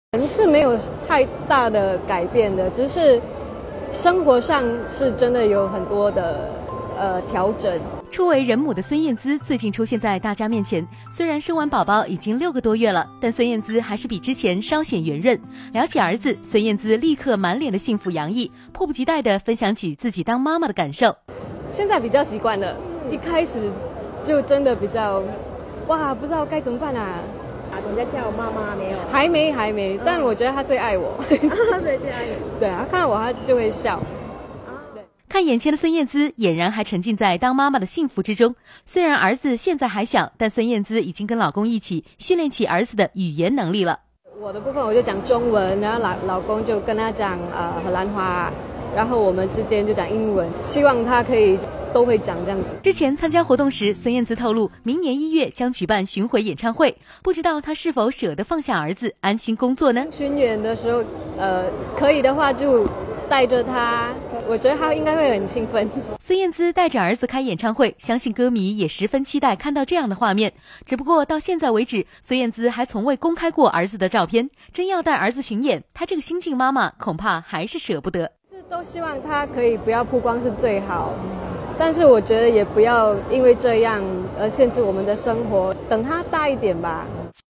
《娱乐资讯》孙燕姿出席活动聊儿子 已经习惯当妈妈
新加坡歌手孙燕姿来港出席代言的奶粉活动，荣升人母的她身材仍然保持得宜，她透露忙于养小朋友，工作方面亦没有怠慢，正筹备明年推出新专辑及亚洲巡回演唱会，其中一站会在香港举行，作为新手妈妈，燕姿表示不知道应该刺激小朋友的兴趣，还是让他自由发发挥，但会细心为他拣选最好的婴儿用品，她坦言宝宝的奶粉全都由大会赞助。